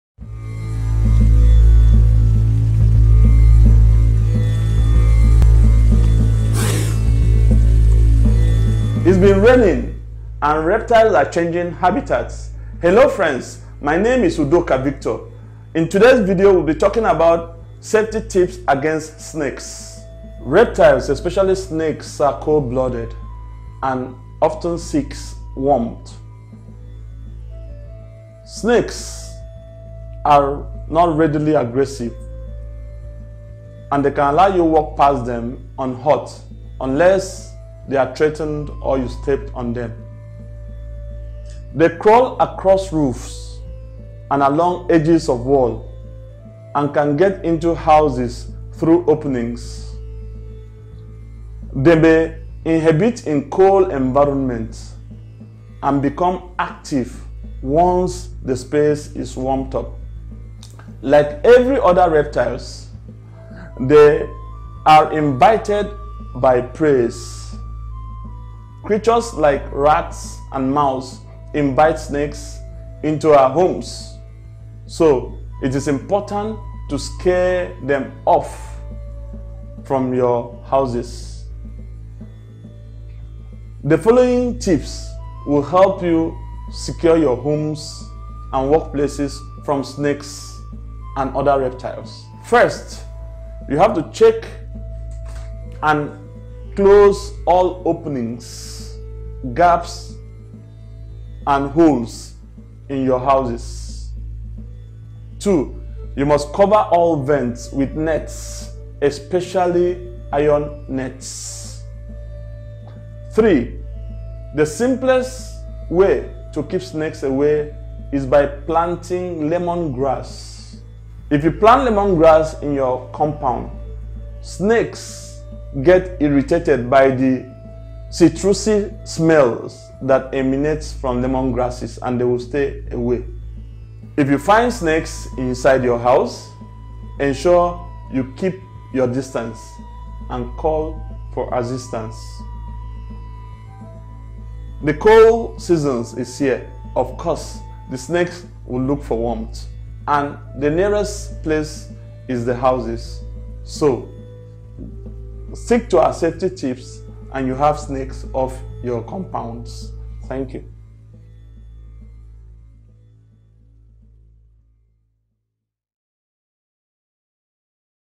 Safety tips against snakes
Snakes and other reptiles pose a serious threat and knowing where snakes might be is only part of preventing snake presence. However, for people with more exposure to snakes, there are other protective measures that can be taken.